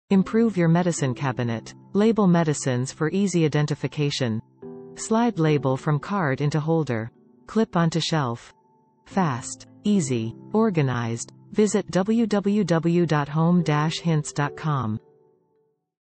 Cabinet Organization - FIRST-AID & PRESCRIPTIONS - Easy. Fast. Organized in less than an hour.
Welcome to Home Hints! Watch this 20-second video showcasing the power of our Cabinet Kit in effortlessly organizing your first-aid and prescription medicines. Say goodbye to medication confusion and hello to a well-organized and easily accessible healthcare hub within your cabinets!

In this video, we introduce the Home Hints Cabinet Kit, designed specifically for cabinet organization. Join us as we demonstrate the simplicity and effectiveness of using Home Hints to transform your cabinet into a functional and organized space for your healthcare needs.

Discover our versatile first-aid and prescription medicine labels, meticulously crafted to address common issues such as allergies, upset stomach, headaches, stuffy nose, and more. These labels make it easy to categorize and locate your medications quickly, ensuring prompt and efficient healthcare management.

Watch as we demonstrate the straightforward process of using the Home Hints Cabinet Kit. First, select the label from the label card. Then, slide the label into the label holder. Finally, effortlessly clip the label holder onto the front of the shelf. It's as simple as 1-2-3!

Experience the benefits of an organized first-aid and medication cabinet: easily find the right remedies, minimize confusion, and promote a safe and well-prepared home. The Home Hints Cabinet Kit enhances the functionality of your cabinets, allowing you to have essential healthcare items at your fingertips.